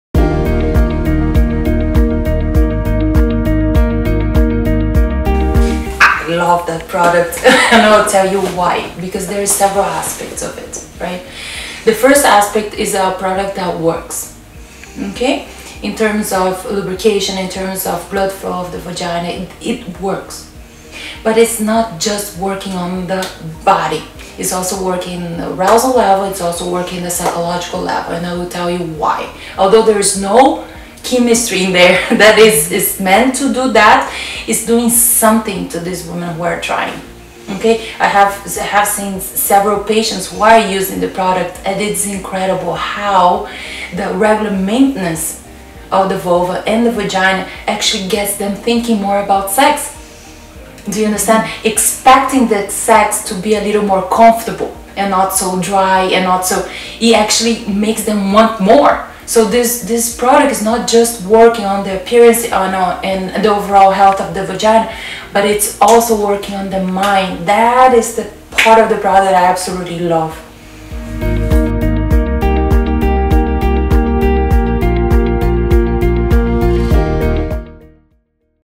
Doctor Testimonial on Co2LiftV, the take-home vaginal treatment by Dr Cristina Sabroso, Sexologist
Hear what Dr. Cristina Sabroso, Clinical Psychologist and Sexologist says how CO2LiftV can help sexual issues